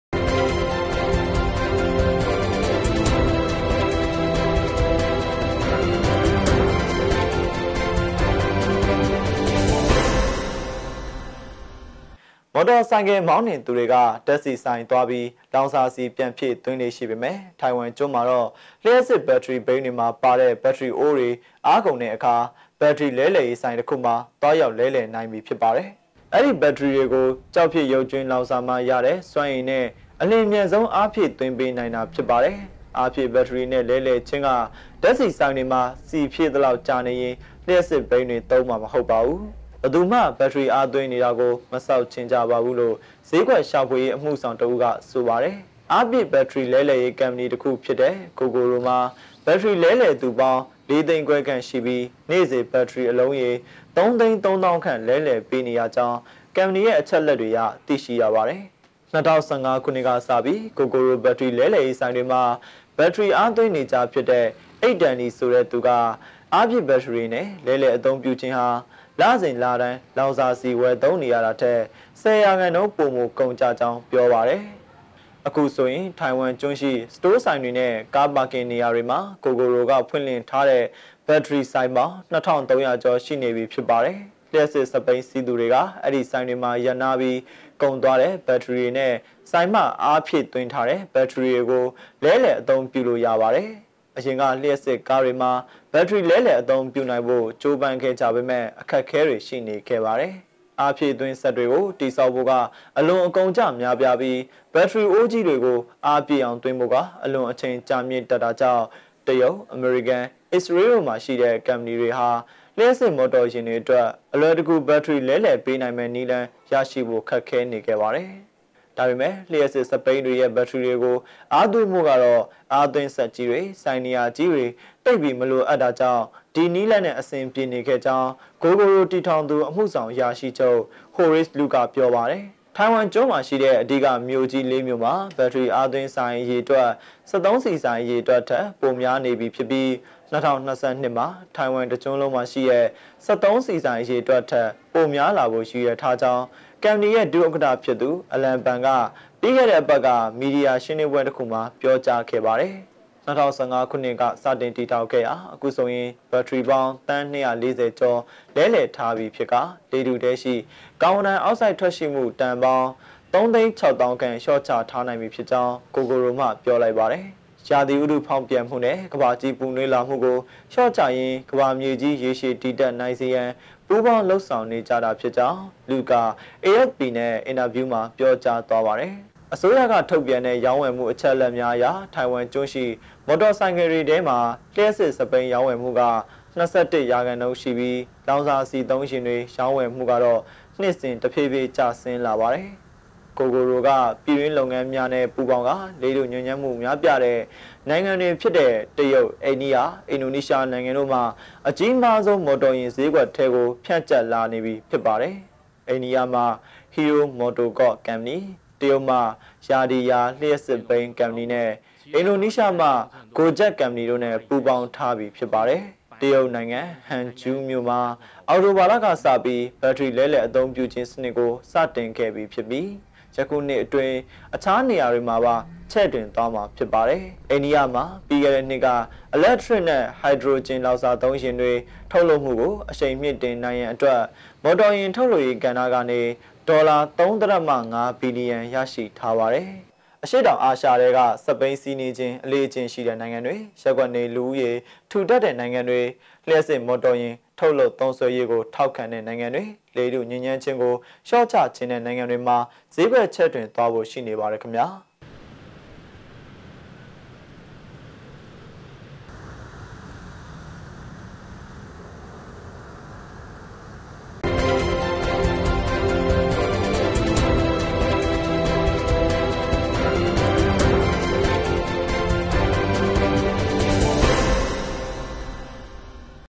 ထိုင်ဝမ်ကျွန်းကလျှပ်စစ်စက်ဘီးအားကုန်ဘက်ထရီများကိုအားဖြည့်ပြီးဘက်ထရီနှင့်ချက်ခြင်းလဲလှယ်အသုံးပြု
မော်တော်ဆိုင်ကယ်မောင်းနှင်သူတွေက ဓာတ်ဆီဆိုင်သွားပြီး လောင်စာဆီပြန်ဖြည့်သွင်းလေ့ရှိပေမယ့် ထိုင်ဝမ်ကျွန်းမှာတော့ လျှပ်စစ်ဘက်ထရီဘီးတွေမှာပါတဲ့ ဘက်ထရီအိုးတွေအားကုန်တဲ့အခါ ဘက်ထရီ လဲလှယ်ရေးဆိုင်တစ်ခုမှာ သွားရောက်လဲလှယ်နိုင်ပြီဖြစ်ပါတယ်။